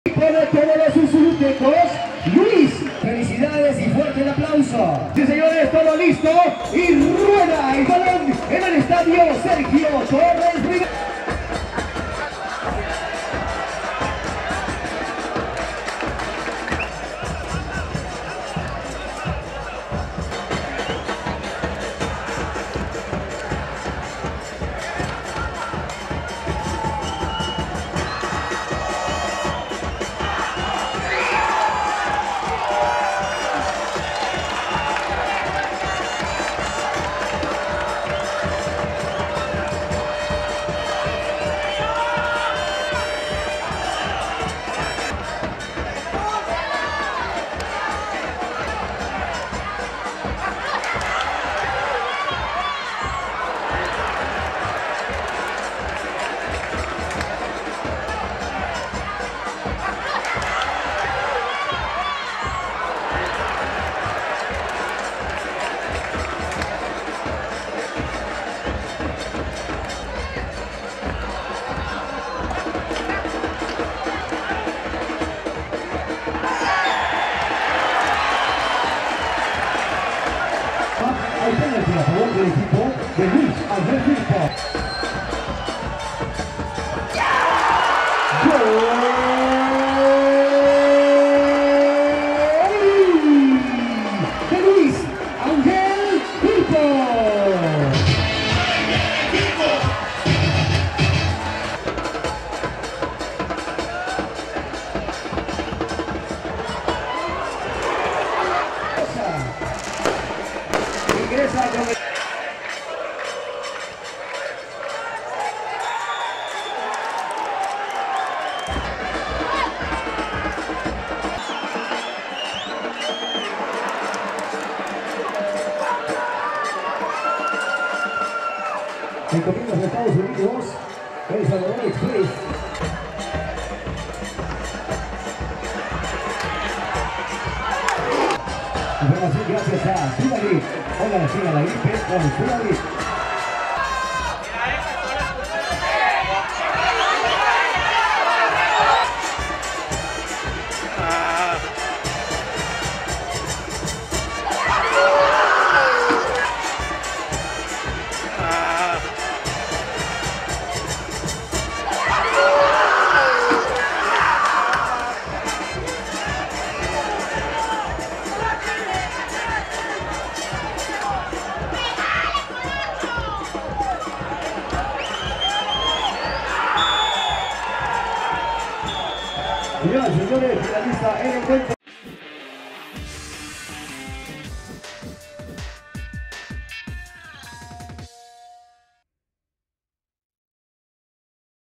RESUMEN l Firpo 1-2 Marte l Jornada 13 l Torneo Clausura 2022.
Firpo perdió en su casa ante los capitalinos del Atlético Marte.